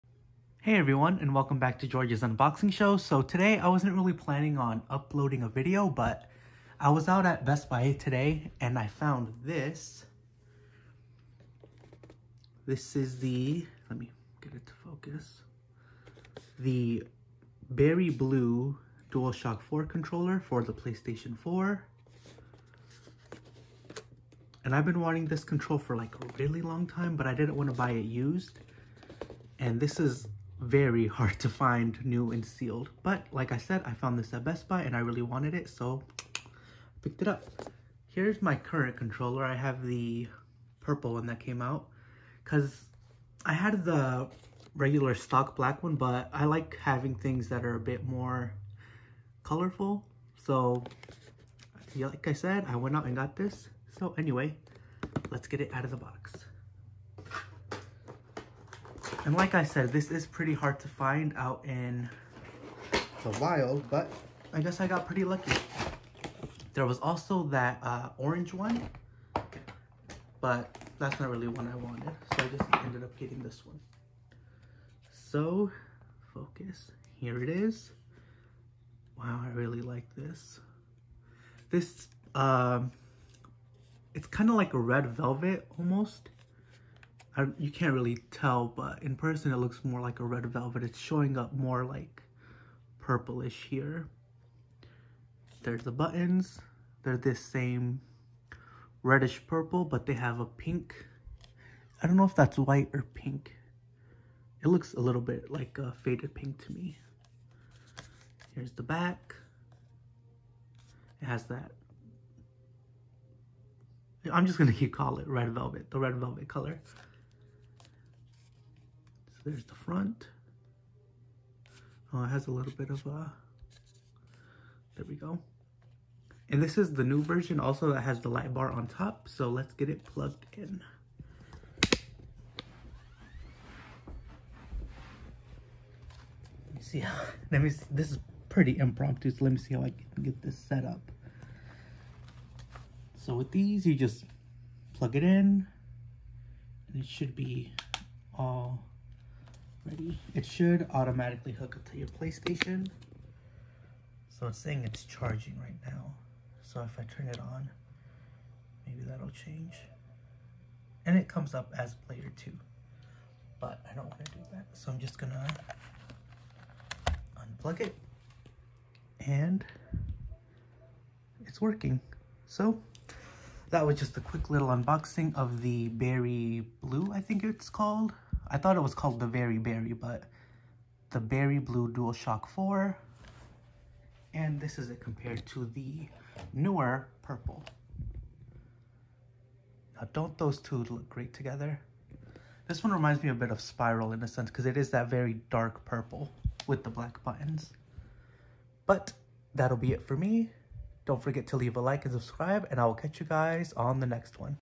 Rare! Berry Blue DualShock4 Unboxing
A quick look at this hard to find controller :)